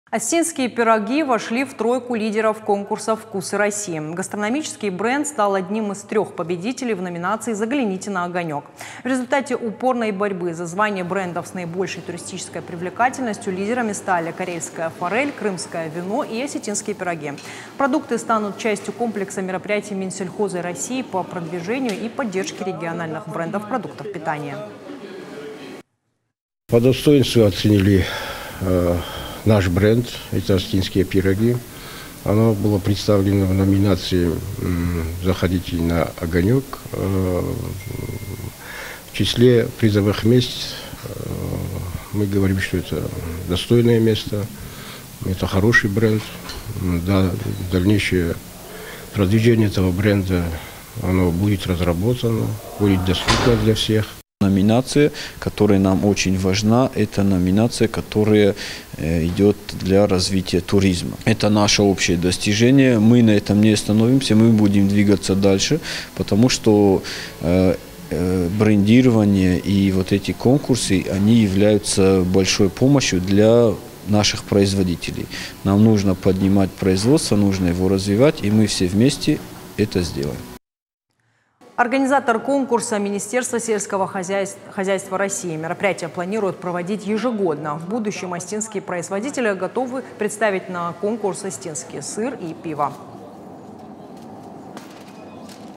Осетинские пироги вошли в тройку лидеров конкурса «Вкусы России»
Осетинские пироги вошли в тройку лидеров конкурса «Вкусы России». Гастрономический бренд стал одним из трех победителей в номинации «Загляните на огонёк».

В результате упорной борьбы за звание брендов с наибольшей туристической привлекательностью лидерами стали карельская форель, крымское вино и осетинские пироги. Продукты станут частью комплекса мероприятий Минсельхоза России по продвижению и поддержке региональных брендов продуктов питания.

Организатор конкурса - Министерство сельского хозяйства России. Мероприятие планируют проводить ежегодно. В будущем осетинские производители готовы представить на конкурс осетинские сыр и пиво.
Мы рядом в:
ok.ru/irystontv